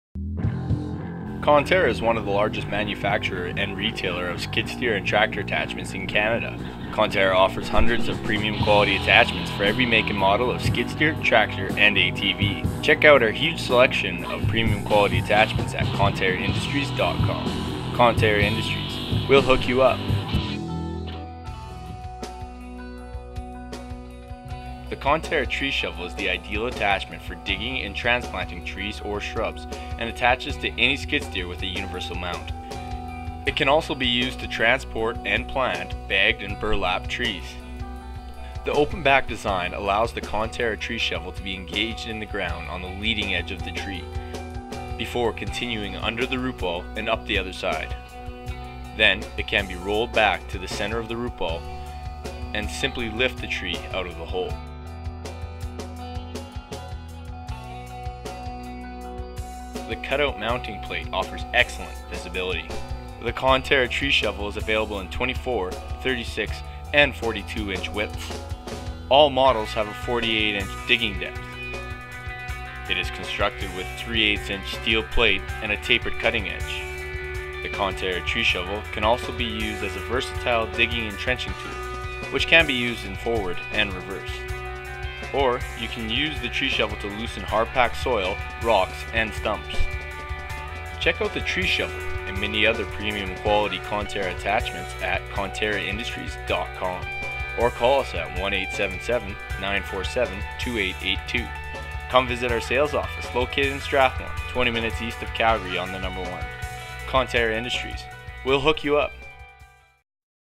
Conterra Tree Shovel for Skid Steers Video
Ideal for digging and transplanting trees and shrubs.  Fits most late model skid steers.  New open back design allows operater to engage the shovel into the ground on the leading edge of the tree then continue all the way under the rootball and up the other side, then roll back to the center of the rootball and simply lift the tree out of the hole.  Transport and plant balled and burlapped trees.  Versatile digging and trenching tool.  Digs and trenches in forward and reverse.  Loosens hard packed soil, rocks and stumps.  Available in 24", 36" and 42" digging widths.  48" digging depth on all models.